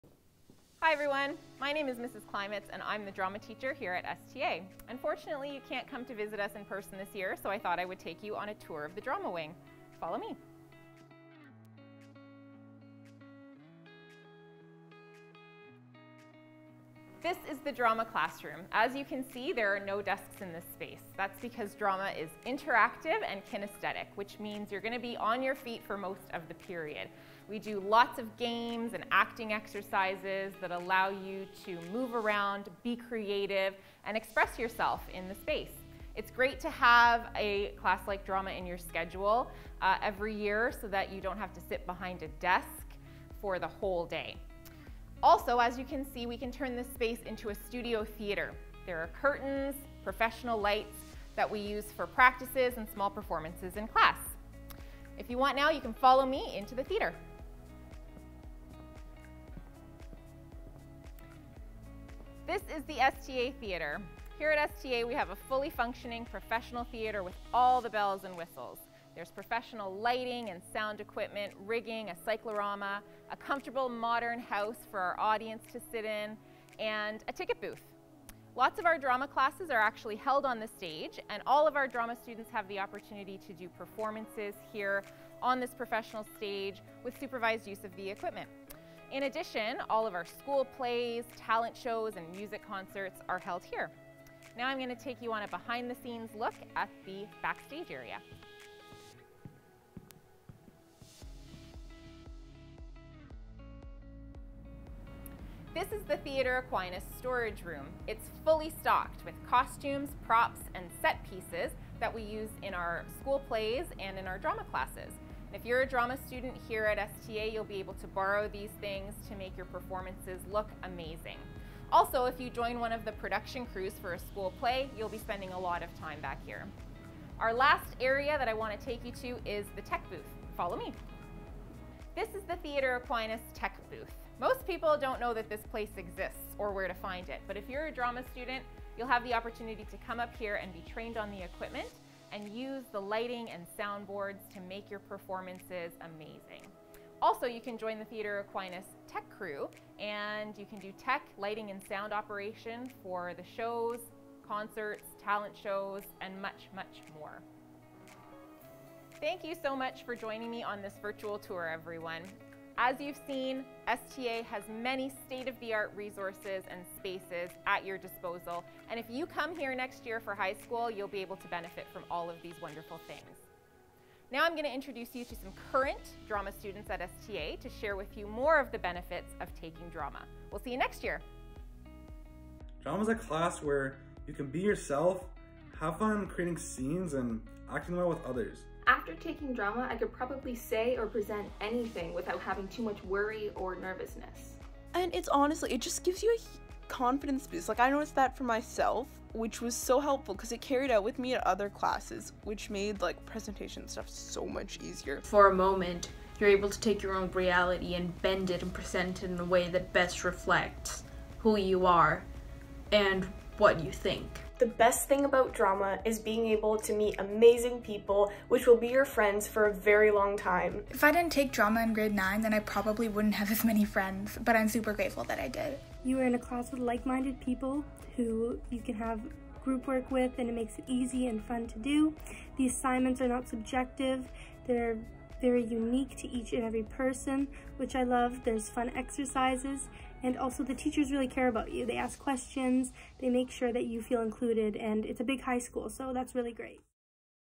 Theatre Aquinas Tour
Mrs. Klimitz takes you on a tour of all that our Drama program has to offer. Learn about some of the highlights of being part of Drama class and Theatre Aquinas productions at STA!